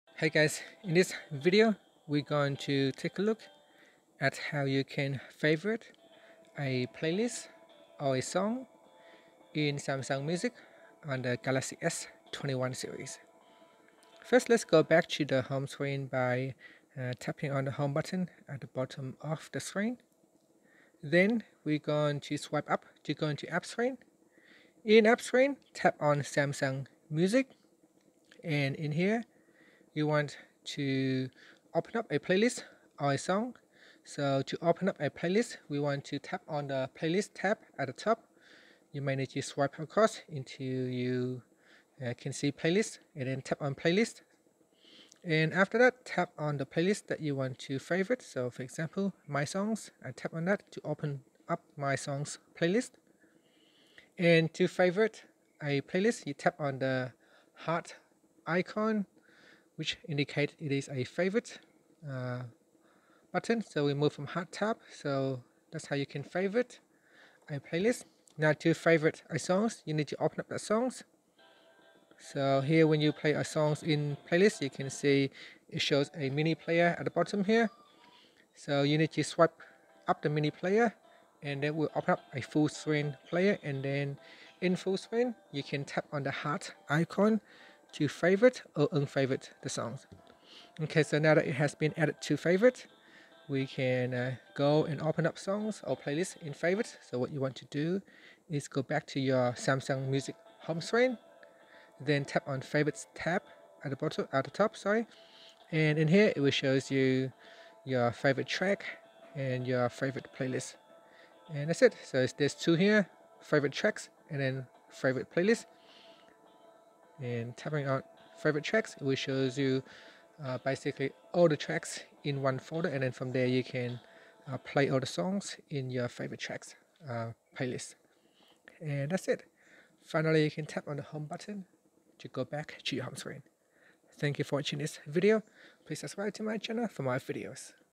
Galaxy S21/Ultra/Plus: How to Favorite a Playlist or Song in Samsung Music
Learn how you can favorite a playlist or song in Samsung Music on the Galaxy S21/Ultra/Plus.